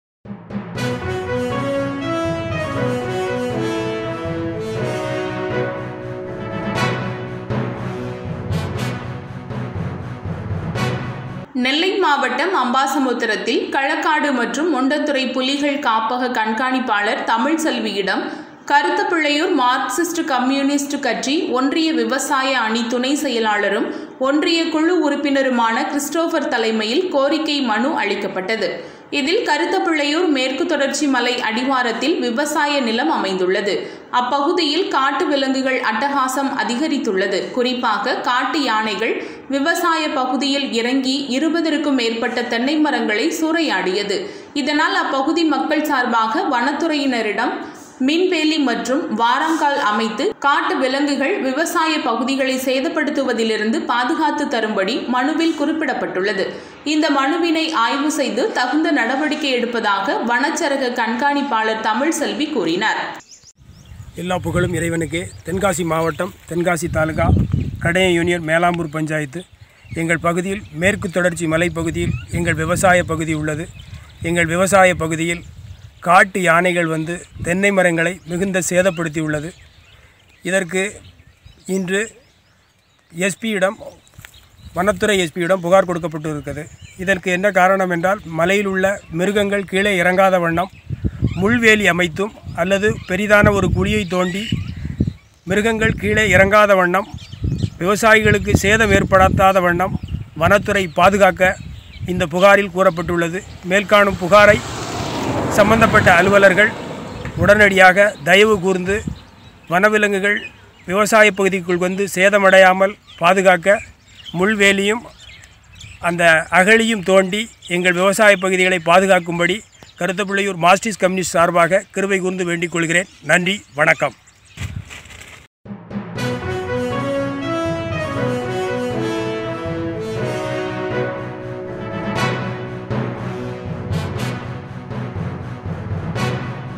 யானைகளின் அட்டகாசம் குறித்து மனு!!!
நெல்லை மார்ச்:05,

நெல்லை மாவட்டம் அம்பாசமுத்திரத்தில் களக்காடு மற்றும் முண்டந்துறை புலிகள் காப்பக கண்காணிப்பாளர் தமிழ் செல்வியிடம் கருத்தபிள்ளையூர் மார்க்சிஸ்ட் கம்யூனிஸ்டு கட்சி ஒன்றிய விவசாய அணி துணை செயலாளரும், ஒன்றிய குழு உறுப்பினருமான கிறிஸ்டோபர் தலைமையில் கோரிக்கை மனு அளிக்கப்பட்டது.
இதில் கருத்தபிள்ளையூர் மேற்கு தொடர்ச்சி மலை அடிவாரத்தில் விவசாய நிலம் அமைந்துள்ளது அப்பகுதியில் காட்டு விலங்குகள் அட்டகாசம் அதிகரித்துள்ளது குறிப்பாக காட்டு யானைகள் விவசாய பகுதியில் இறங்கி இருபதிற்கும் மேற்பட்ட தென்னைமரங்களை சூரையாடியது இதனால் அப்பகுதி மக்கள் சார்பாக வனத்துறையினரிடன் மின் வேலி மற்றும் வாராங்கள் அமைத்து காட்டு விலங்குகள் விவசாய பகுதிகளை சேதப்படுத்துவதிலிருந்து பாதுகாத்து தரும் படி மனுவில் குறிப்பிட பட்டுள்ளது . இந்த மனுவினை ஆய்வு செய்து தகுந்த நடவடிக்கை எடுப்பதாக வனச்சரக கண்காணிப்பாளர் தமிழ் செல்வி கூறினார்.